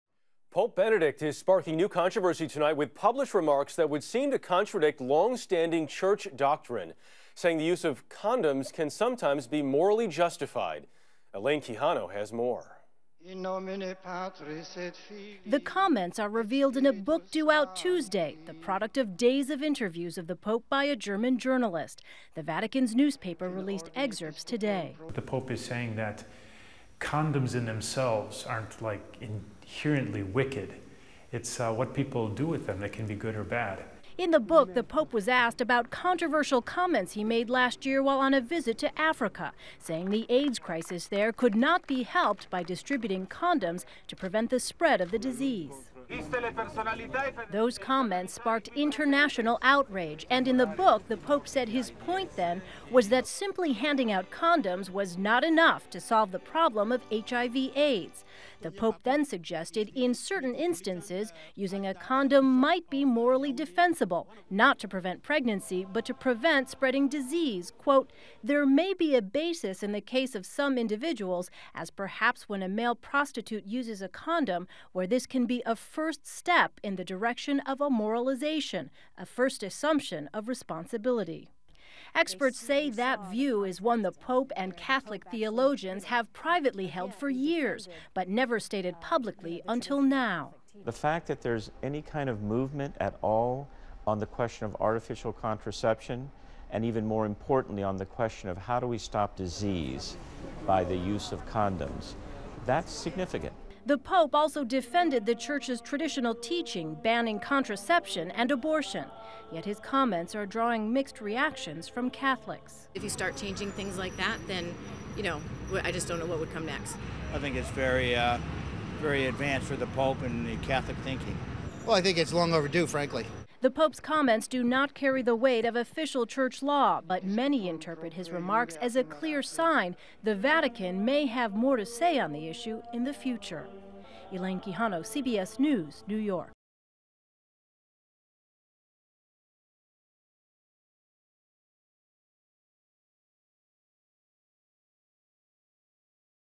Pope: In Some Cases, Condoms Can Be Morally Justified
In a new book, Pope Benedict said in some limited cases using condoms might be morally justified to prevent spreading diseases like HIV or AIDS. Elaine Quijano reports.